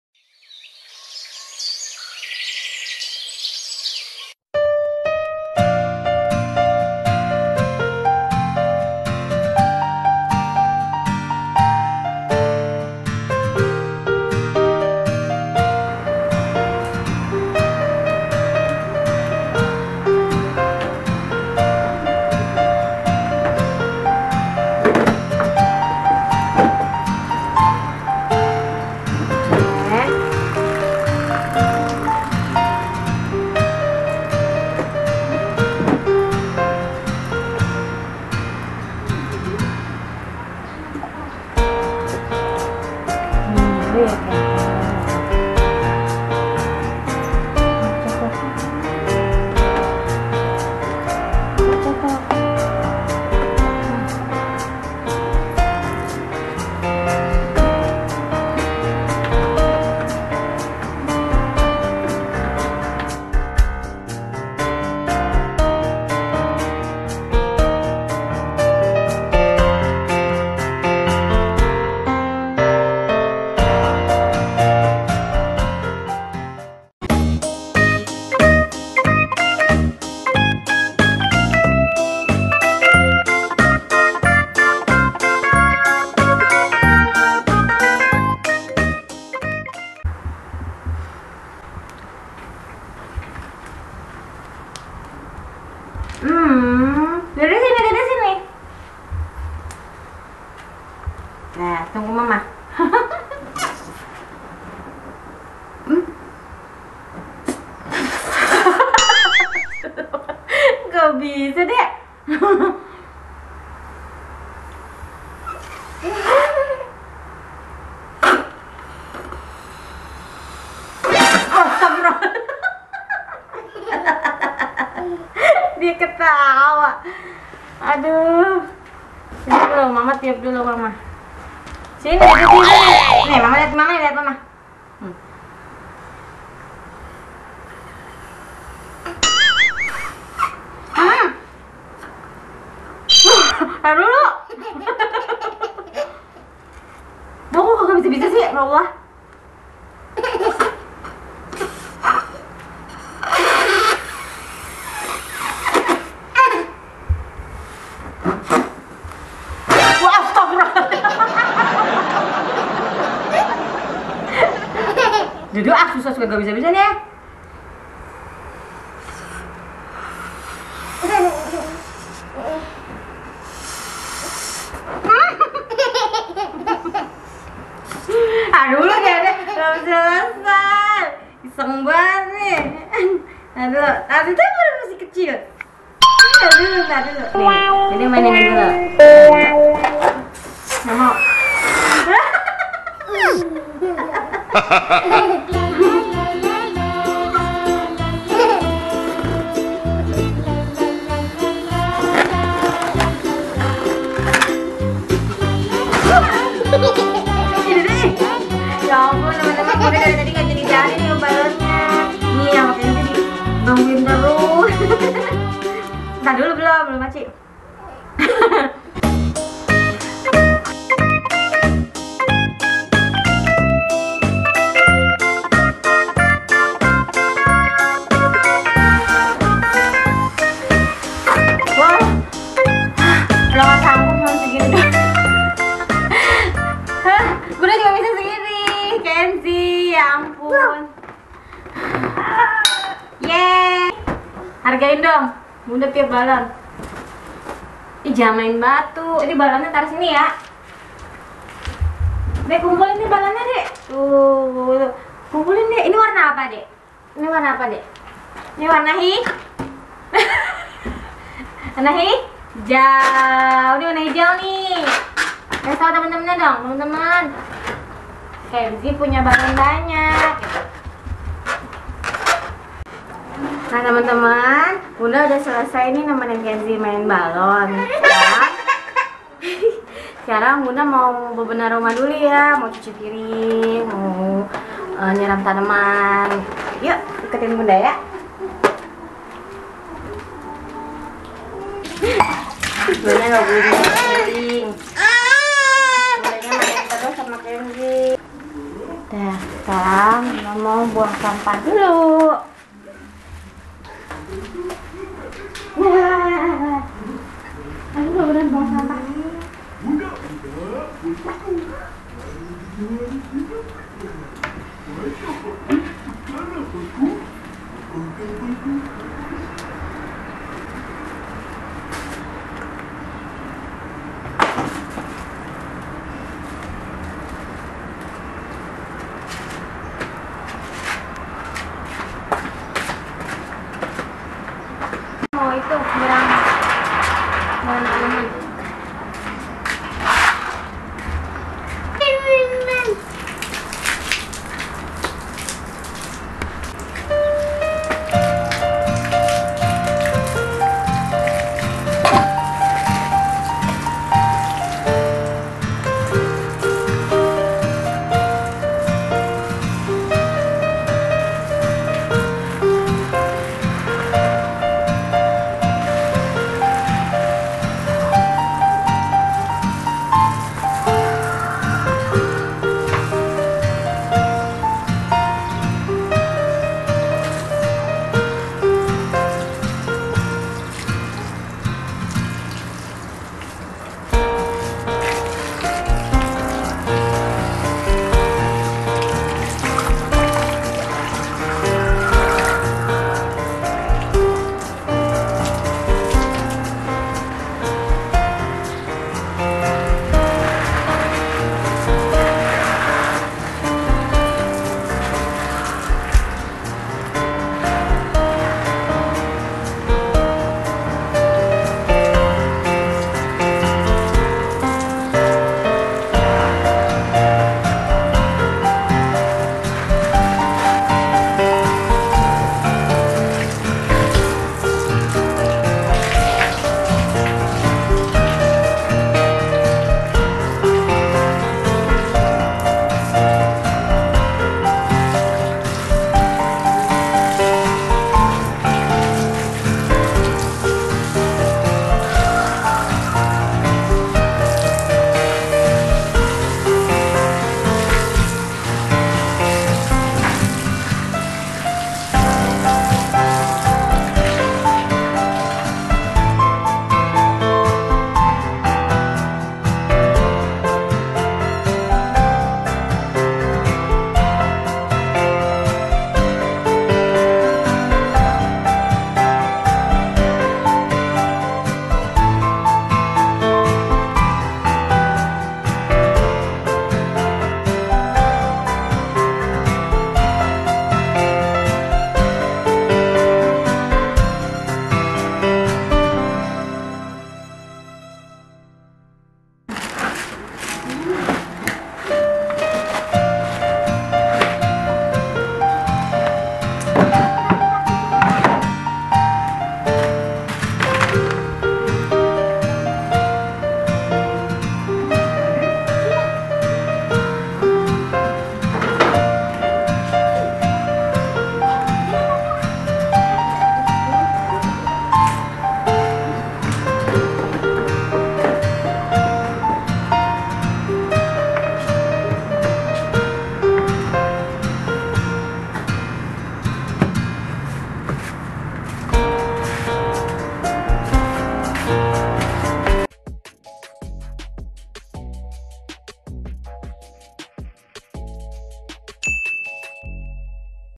KESEHARIAN IBU RUMAH TANGGA - BERSIH BERSIH RUMAH HINGGA BERMAIN BERSAMA ANAK
Hallo teman teman semua pagi ini bunda mau bersih bersih rumah,menyiram tanaman,mencuci piring,sambil bunda bermain balon bersama kenzi  😁.
Tonton video nya sampai habis ya
Happy Watching 🤗